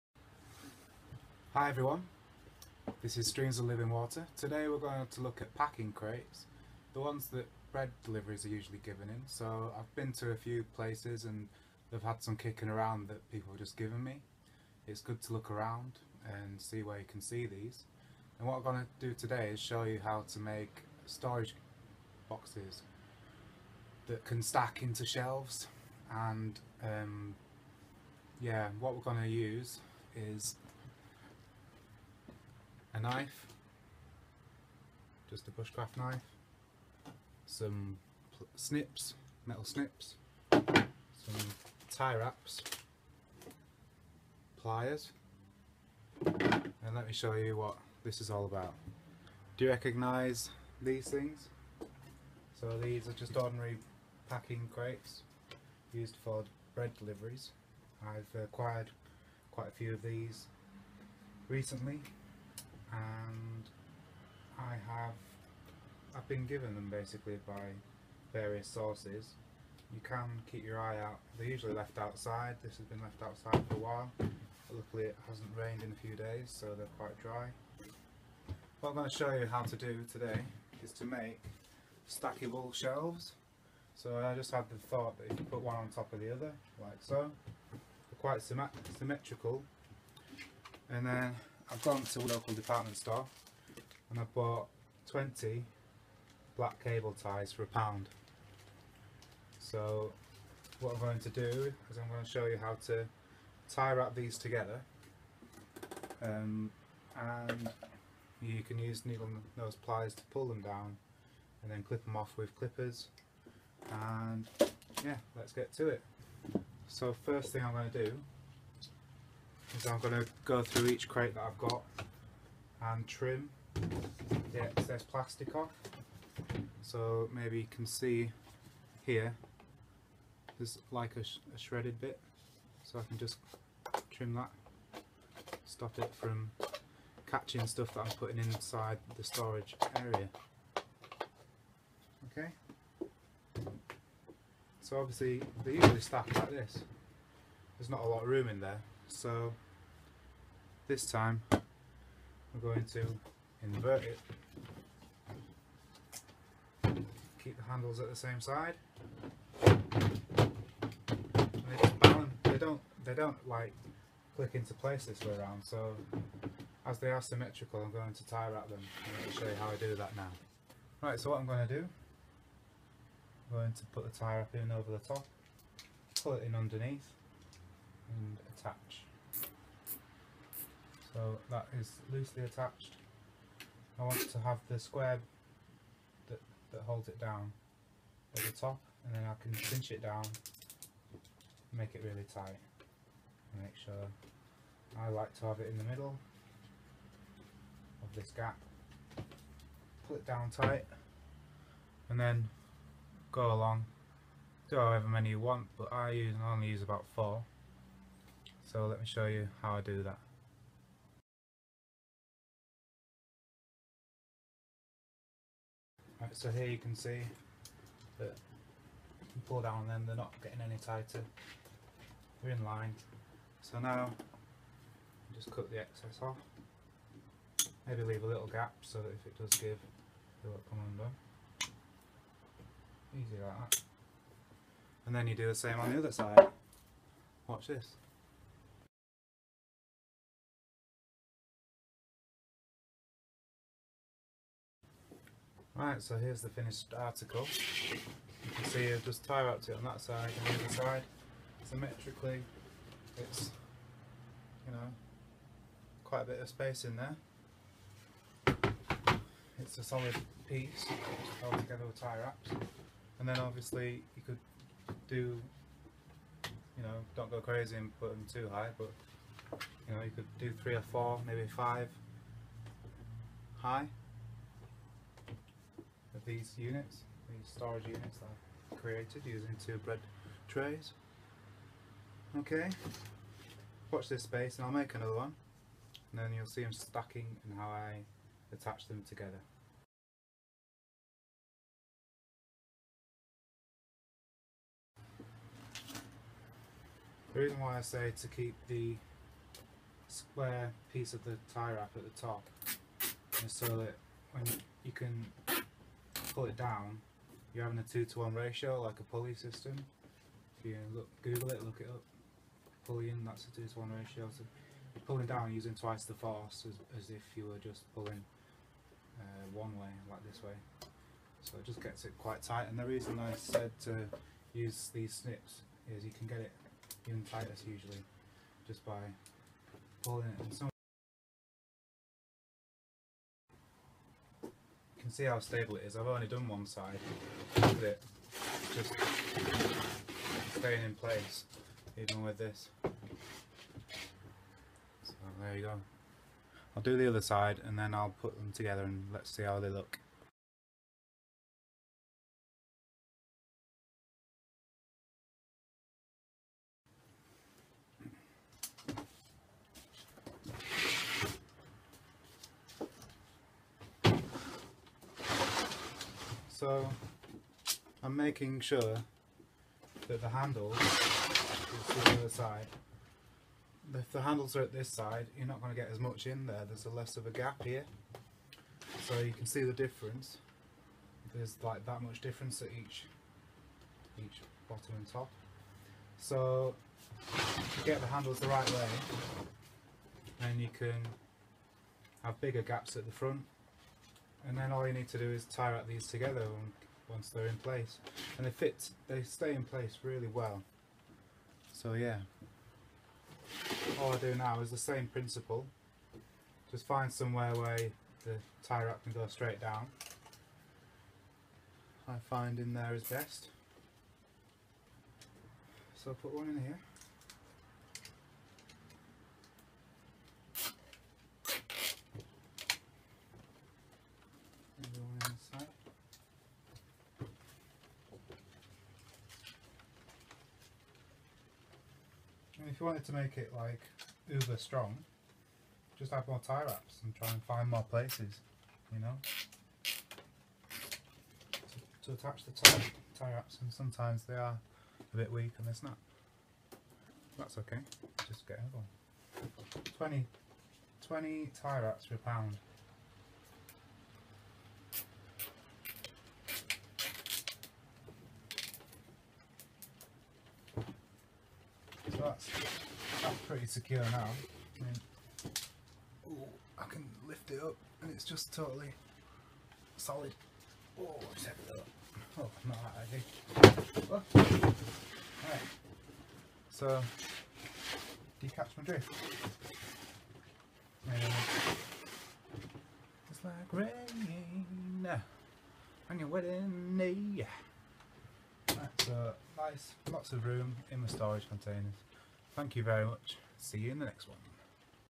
Bread Crates or Trays Recycled to make Stacking Shelving System using Tie Wraps
Made these stacking shelving system out of old bread crates and tie wraps.  Dimensions for each shelf unit (2 bread trays) 550mm x 670mm x 370mm